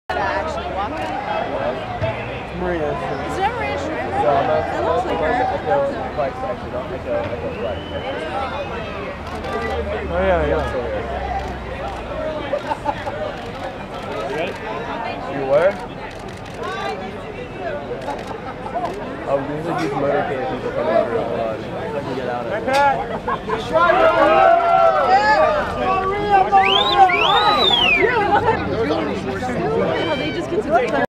Maria Shriver, Patrick Christopher and Katherine Schwarze...
Maria Shriver, Patrick Christopher and Katherine Schwarzenegger depart the Rolling Stones Surprise Club Gig in Los Angeles, 04/.27/13